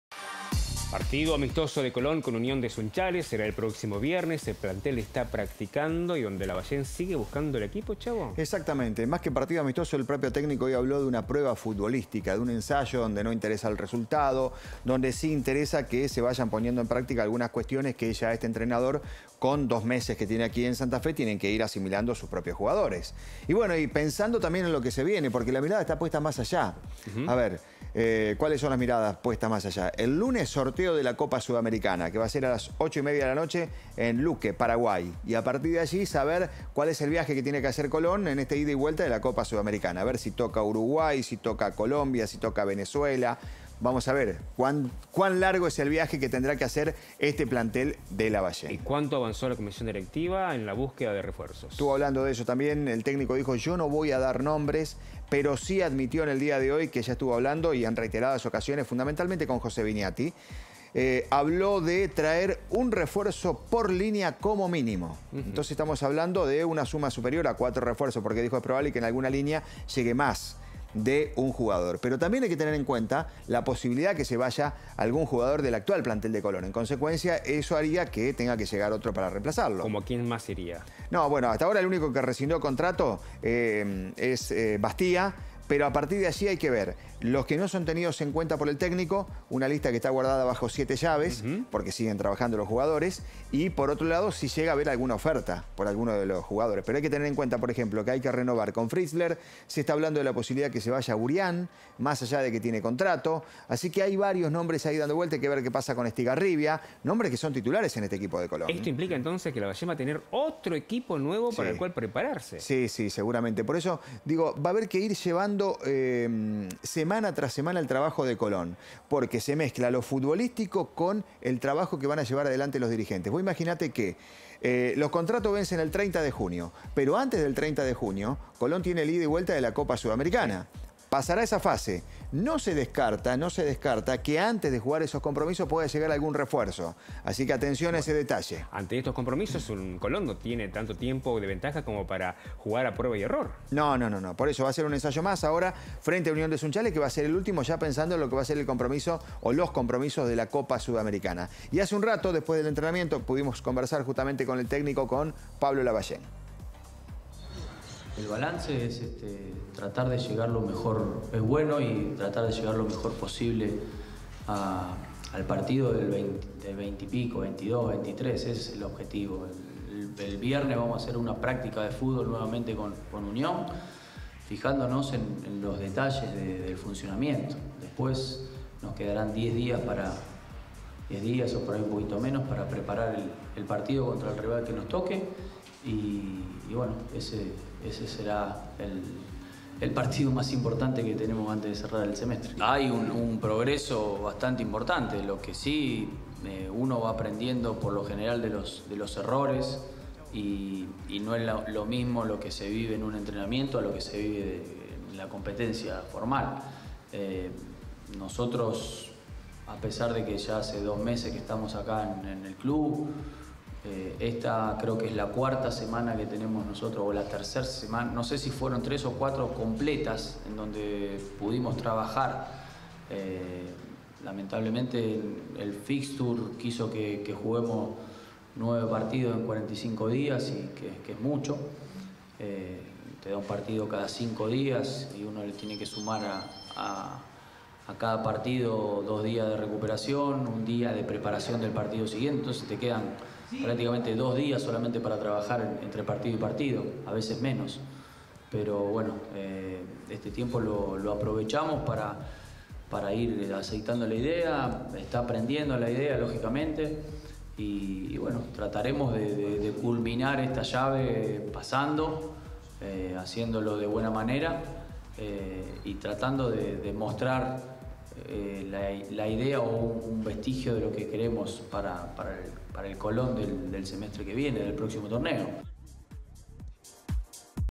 Colón espera rival por la Copa Sudamericana y define refuerzos
El plantel sabalero sigue trabajando a las órdenes de Pablo Lavallén. A la espera de conocer el rival por la Copa Sudamericana, el sabalero jugará otro cotejo amistoso ante Unión de Sunchales este viernes en el Estadio Centenario